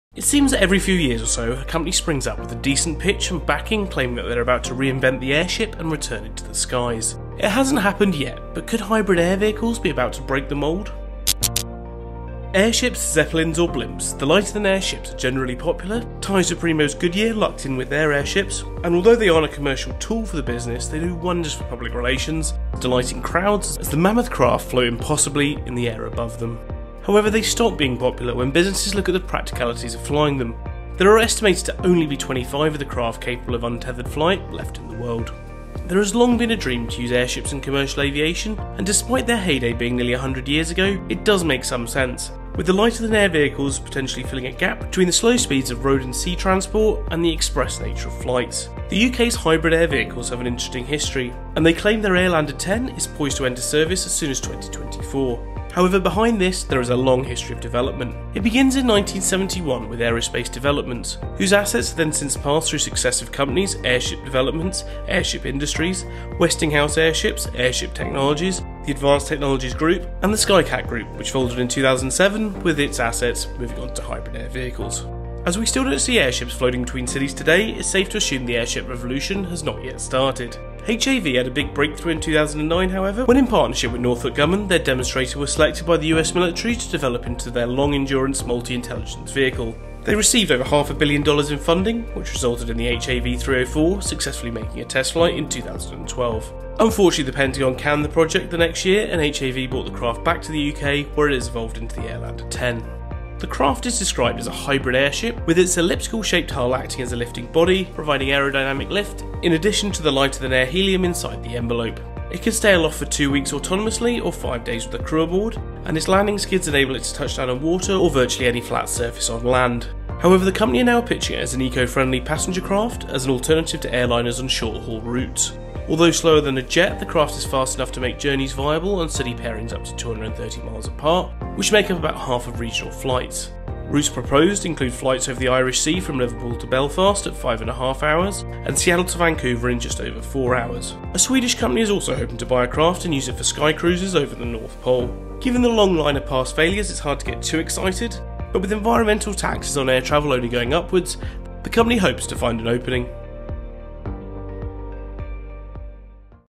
Will Airships Make It This Time?
It seems that every few years or so, a company springs up with a decent pitch and backing claiming that they are about to reinvent the airship and return it to the skies. It hasnt happened yet, but could hybrid air vehicles be about to break the mold?